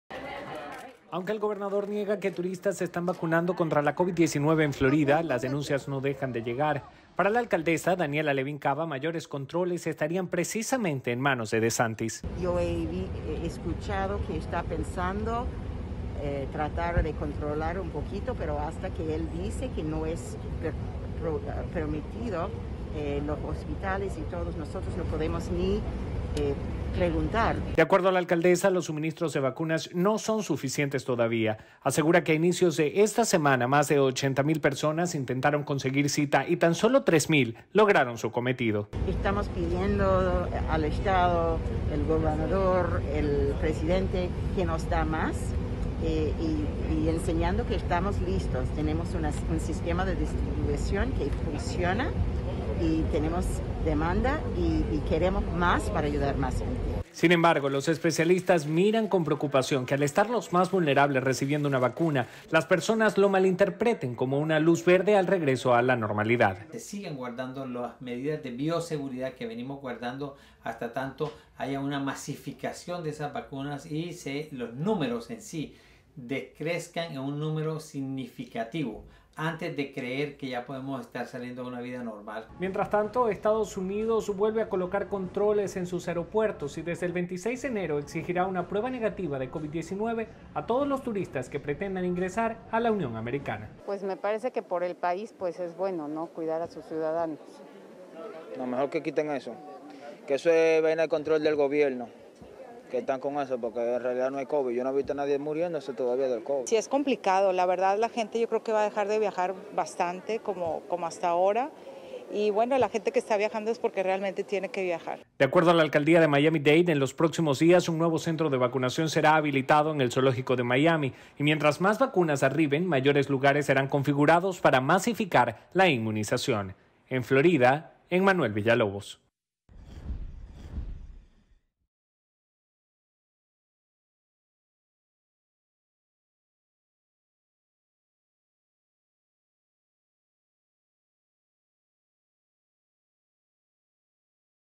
Control en el acceso a vacunas recae en el gobernador de Miami
Ante las denuncias de que turistas se estarían vacunando en Miami contra la covid-19, aun cuando en esta fase solo se permite a residentes de la tercera edad inmunizarse, la alcaldesa de Miami-Dade, Daniella Levine Cava, aseguró que el control de las dosis está en manos del gobernador Ron DeSantis.

Mientras tanto, las medidas de prevención tocan hoy precisamente a muchos turistas que deberán presentar ahora una prueba negativa PCR antes de ingresar al país.

Reporta: Enmanuel Villalobos